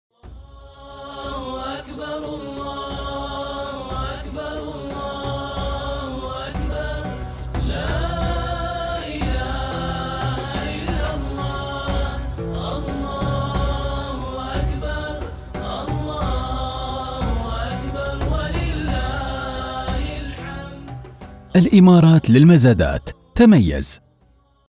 عيد الأضحى المبارك
الإمارات للمزادات تهنئكم بمناسبة عيد الأضحى المبارك، كل عام وأنتم بخير.